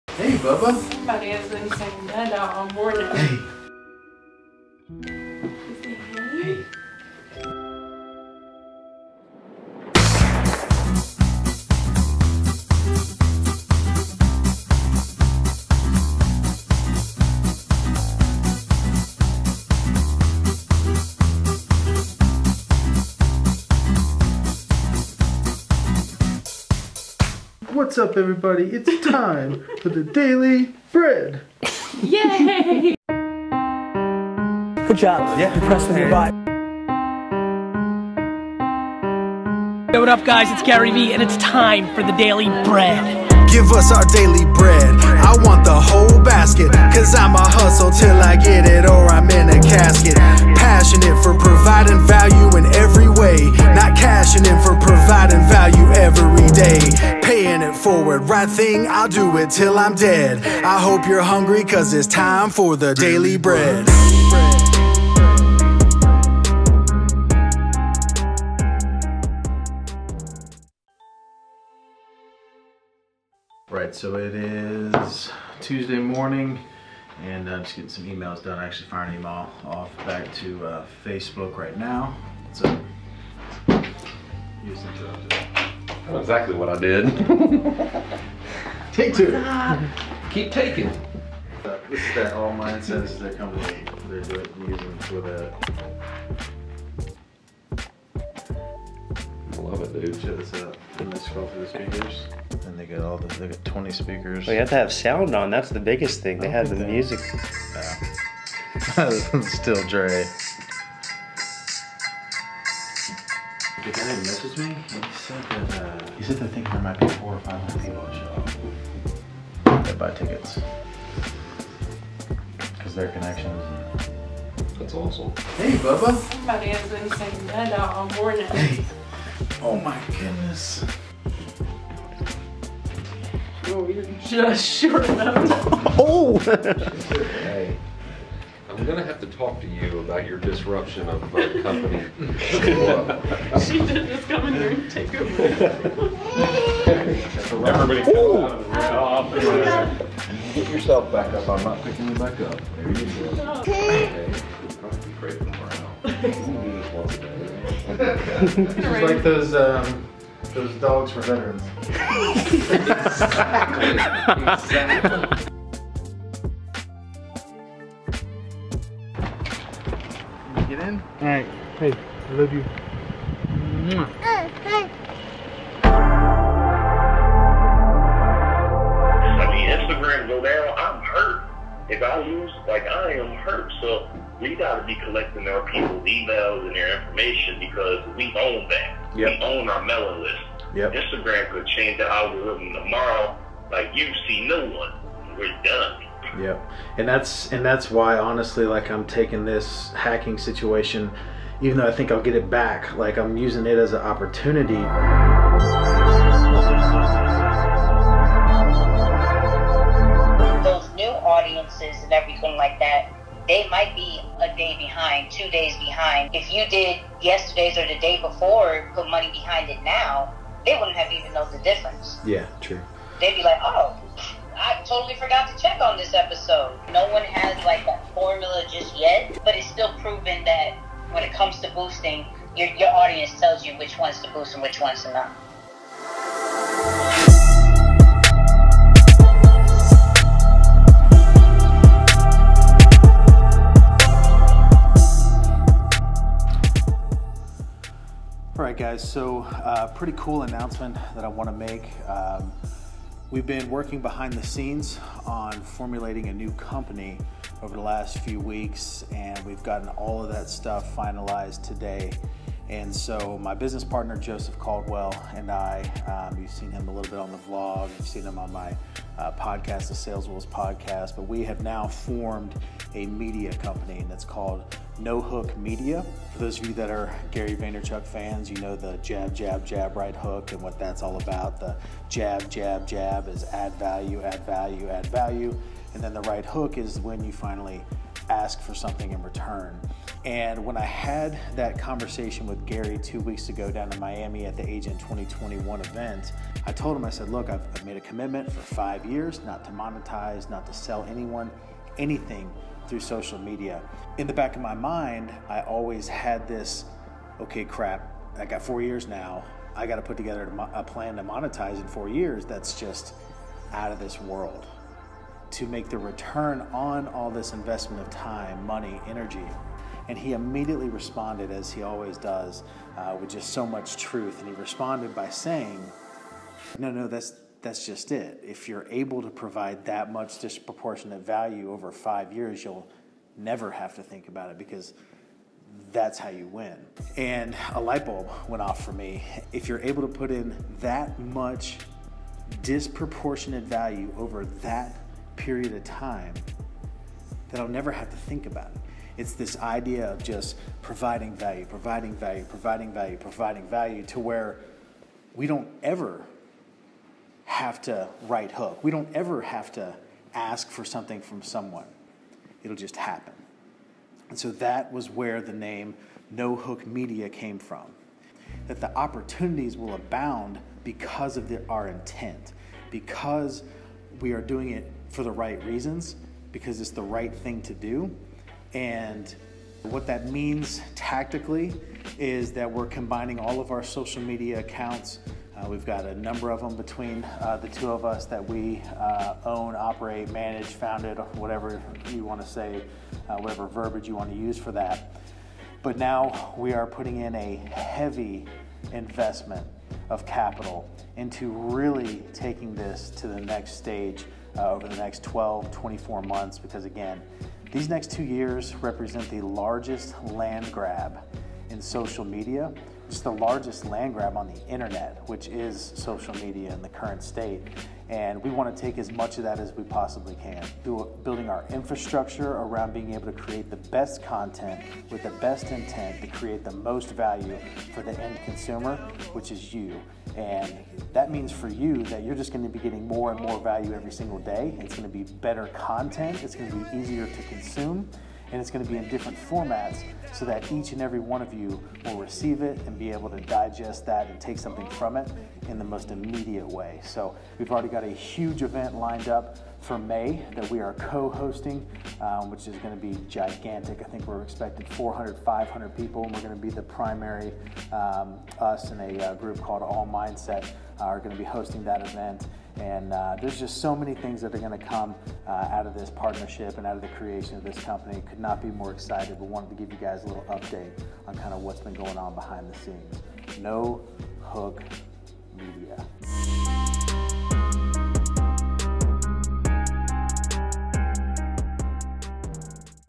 No Hook || The Daily Bread || Ep. 27
In this episode, we launch No Hook Media, creating the infrastructure to scale VALUE. Oh, and Tyler's daughter stops by the office and steals the show!